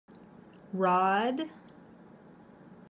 How to pronounce the rod/rɑːd/ sound in American IPA
Learn how to pronounce rod .
Học cách phát âm chuẩn của từ rod/rɑːd/ trong tiếng Anh-Mỹ. Để học và luyện tập cách phát âm miễn phí, bạn vui lòng truy cập